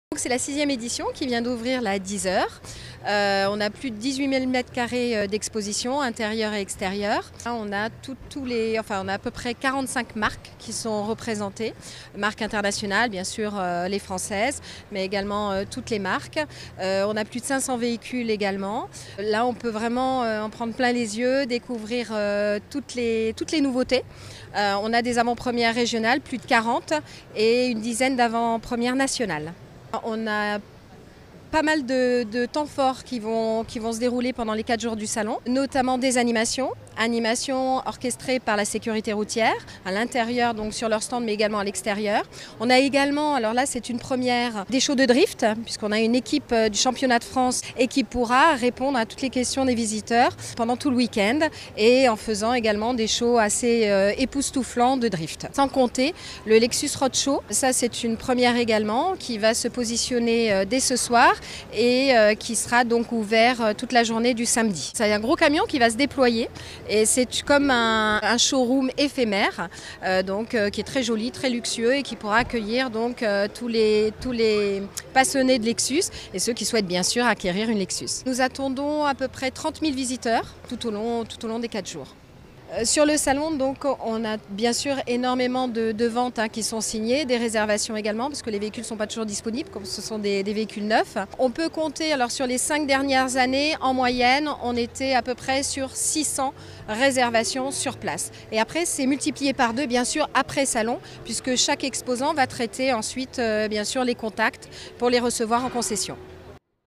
C'est parti pour 4 jours de salon de l'auto à Chanot
L'événement, organisé par le groupe La Provence, est devenu incontournable au fil des ans. Pour la sixième édition du Salon de l'auto Marseille Provence, c'est 45 marques qui ont fait le déplacement pour proposer plus de 500 modèles aux visiteurs. L'occasion d'essayer et pourquoi pas de craquer pour un nouveau véhicule.